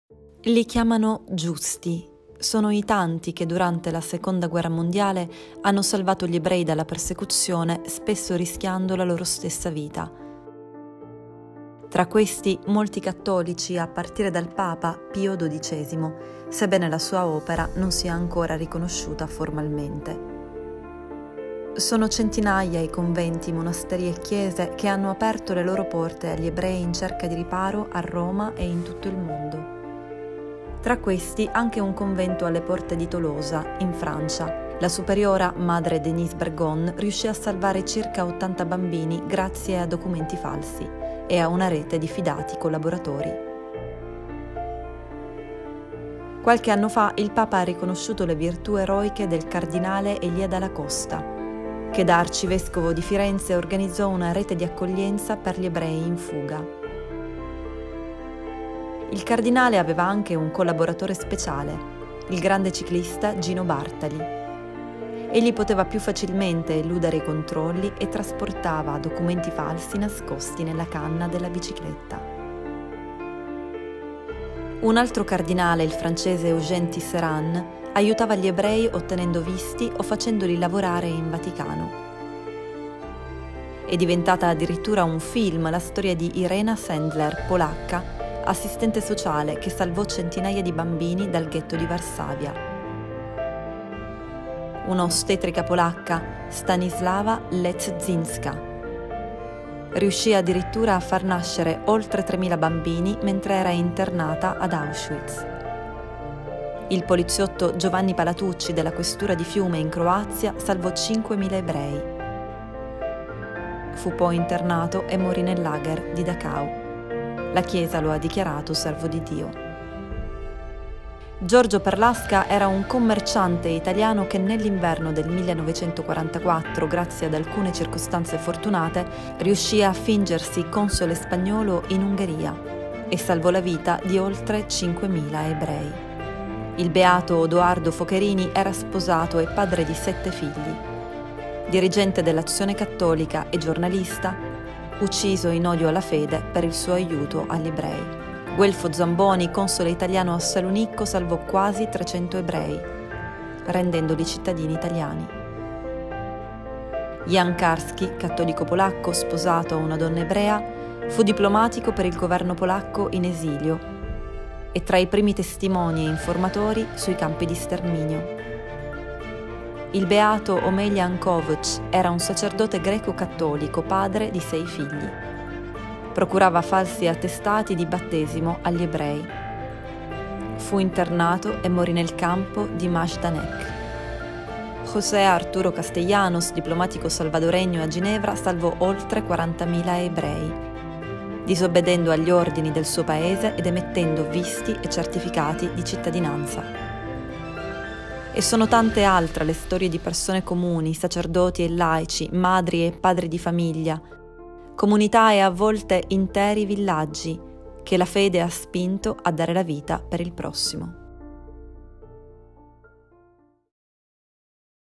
Giorno della Memoria: i “giusti” cattolici
Il 27 gennaio ricorre il Giorno della Memoria e sono tanti i cattolici che, durante la II Guerra Mondiale, hanno salvato gli ebrei dalla persecuzione…

🔵 Scopri chi sono in questo video 🔵